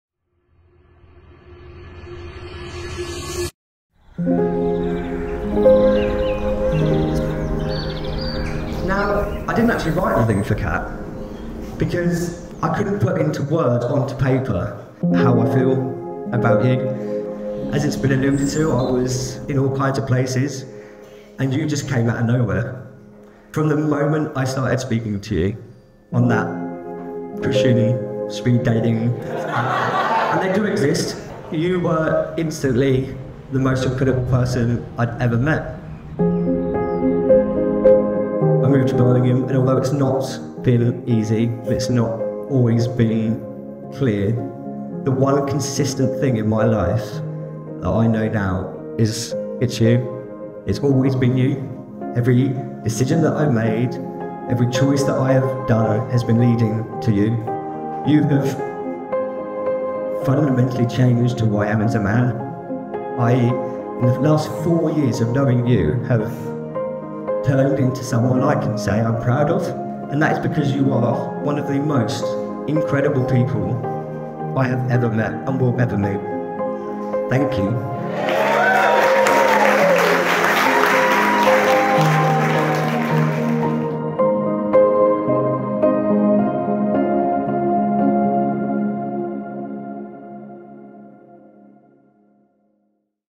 "It's you, it's always been you..." | KATERINA & DANIEL | COTSWOLDS WEDDING | Teaser Trailer
"It's always been you..."

One of the most heart warming and beautiful speeches I've ever heard on a wedding day, and this is just a small snippet of it!

Can't wait to show more of the full highlights film in the coming weeks :)